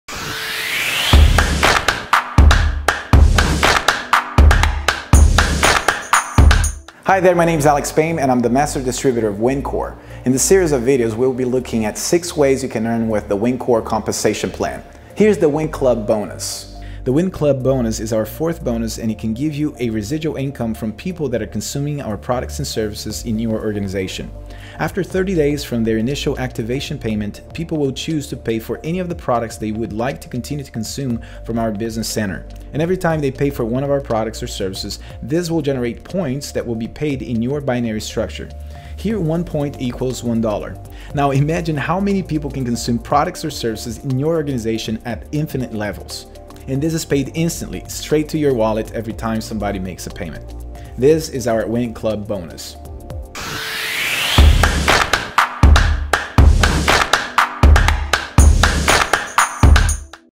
WINKORE Winklub Bonus Explained
The first and only fully DECENTRALIZED system that distributes affliate commissions in real time without ever touching anyone’s money, through a Hybrid Intelligent Binary System that pays instant commissions right to your wallet.